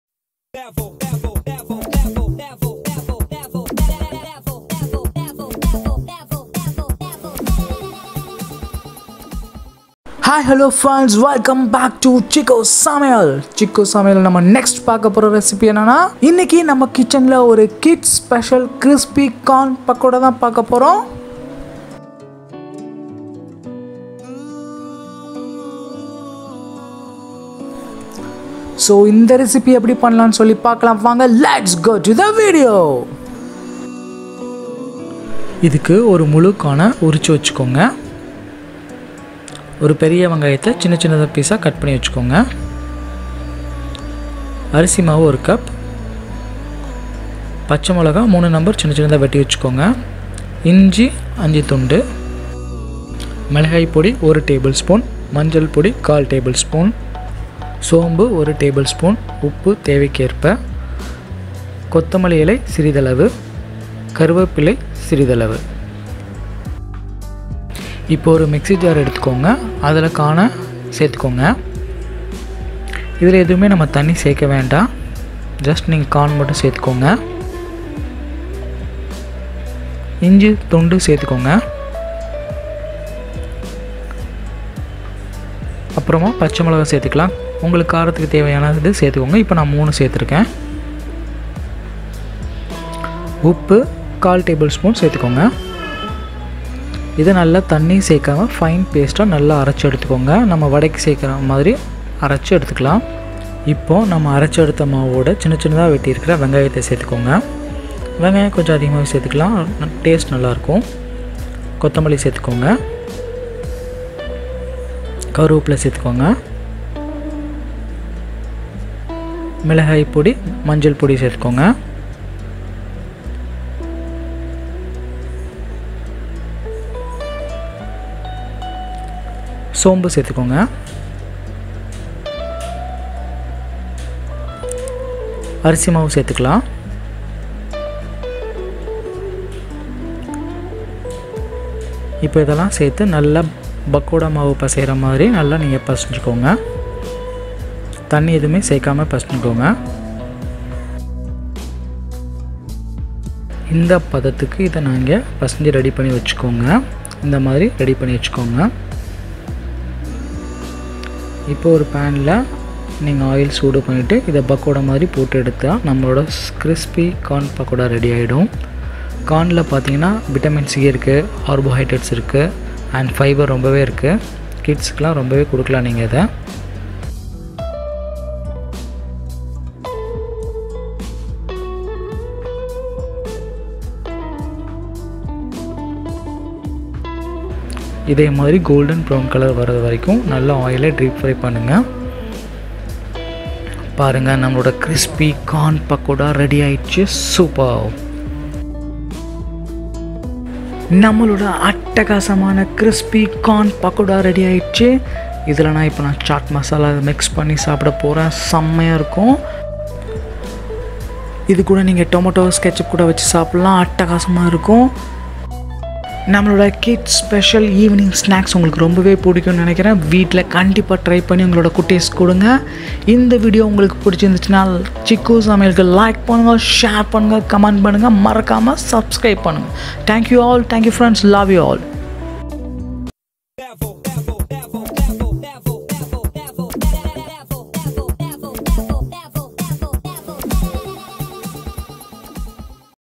Crispy 🌽Pakoda in Tamil🥰/ Kids Spl Evening Snacks/Healthy Recipe🥳
HELLO EVERYONE….
IN THIS VIDEO I AM GOING TO SHARE A SNACK RECIPE WITH KIDS FAVOURITE CRISPY CORN PAKODA IN TAMIL.DO TRY IT IN THESE COOL EVENING WITH TEA AND ITS VERY EASY TO MAKE.
 FOLLOW OUR CHANNEL
COOKING  BY KUMAR
VIDEO SHOOTING BY CHITRA
EDITING BY  CHITRA & KUMAR